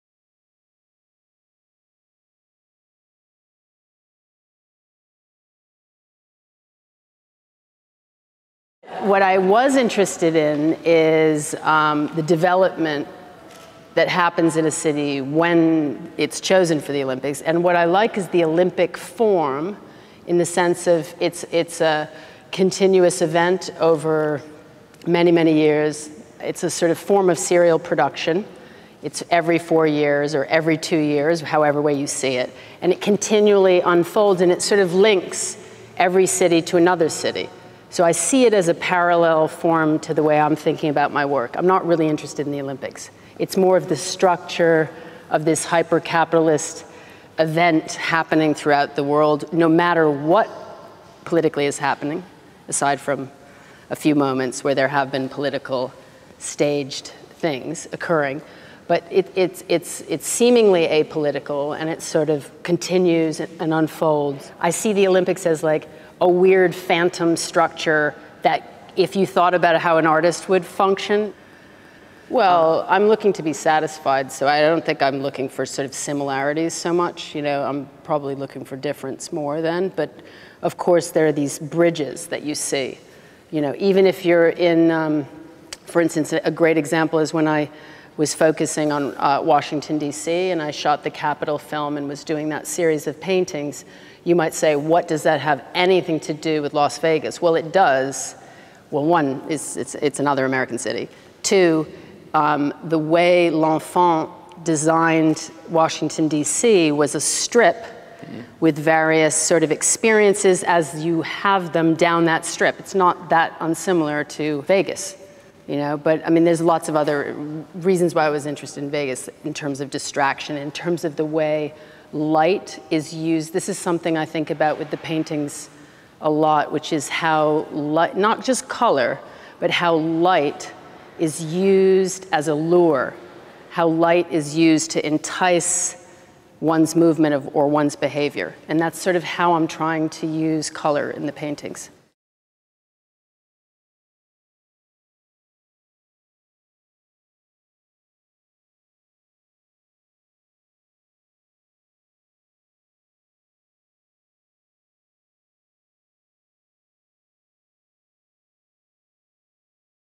In the Gallery: Sarah Morris on 'Bye Bye Brazil' | White Cube
Sarah Morris discusses her 2013 exhibition at White Cube Bermondsey, 'Bye Bye Brazil', featuring her film 'Rio' along with paintings and works on paper.